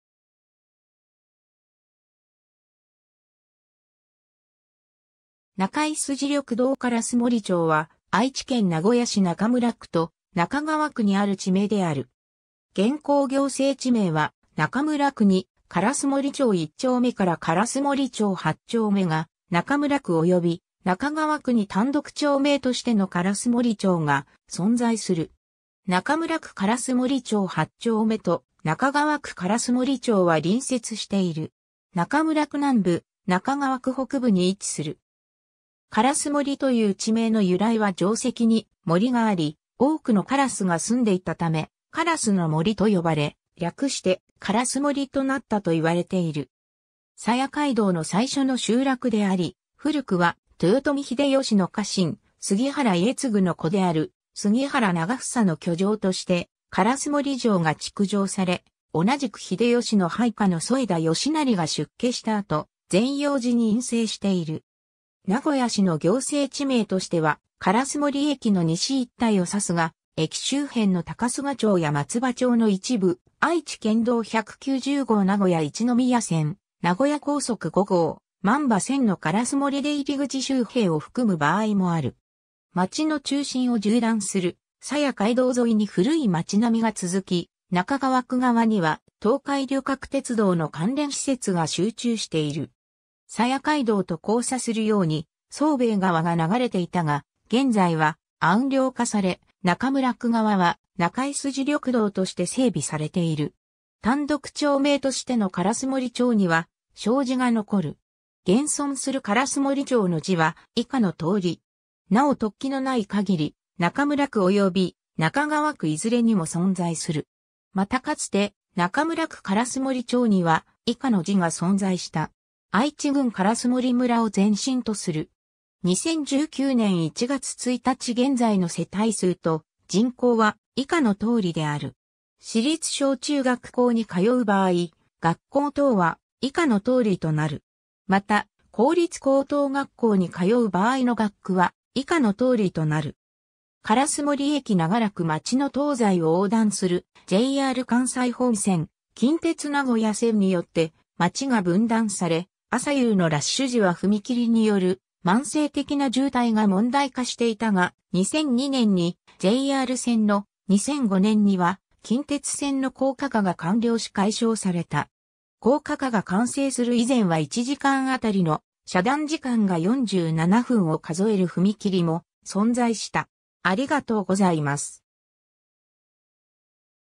烏森町
中川区の町・字
中村区の町・字
中井筋緑道 烏森町（かすもりちょう）は、愛知県名古屋市中村区と中川区にある地名である。
現行行政地名は中村区に烏森町1丁目から烏森町8丁目が、中村区及び中川区に単独町名としての烏森町が存在する（いずれも住居表示未実施地区）。
中村区烏森町8丁目と中川区烏森町は隣接している。
 中村区南部、中川区北部に位置する。
烏森という地名の由来は城跡に森があり、多くの烏が棲んでいたため「烏（からす）の森」と呼ばれ、略して「烏森（かすもり）」となったと言われている。
佐屋街道の最初の集落であり、古くは豊臣秀吉の家臣、杉原家次の子である杉原長房の居城として烏森城が築城され、同じく秀吉の配下の副田吉成が出家した後、禅養寺に隠棲している。
 名古屋市の行政地名としては烏森駅の西一帯を指すが、駅周辺の高須賀町や松葉町の一部、愛知県道190号名古屋一宮線、名古屋高速5号万場線の烏森出入口周辺を含む場合もある。
町の中心を縦断する佐屋街道沿いに古い街並みが続き、中川区側には東海旅客鉄道（JR東海）の関連施設が集中している。
佐屋街道と交差するように惣兵衛川（中井筋）が流れていたが、現在は暗梁化され中村区側は中井筋緑道として整備されている。
 単独町名としての烏森町には小字が残る。
現存する烏森町の字は以下の通り。
なお特記のない限り、中村区及び中川区いずれにも存在する。
 またかつて中村区烏森町には以下の字が存在した。
 愛知郡烏森村を前身とする。
 2019年（平成31年）1月1日現在の世帯数と人口は以下の通りである。
 市立小・中学校に通う場合、学校等は以下の通りとなる。
また、公立高等学校に通う場合の学区は以下の通りとなる。
 烏森駅 長らく町の東西を横断するJR関西本線、近鉄名古屋線によって町が分断され、朝夕のラッシュ時は踏切による慢性的な渋滞が問題化していたが、2002年にJR線の、2005年には近鉄線の高架化が完了し解消された。
高架化が完成する以前は1時間当たりの遮断時間が47分を数える踏切も存在した。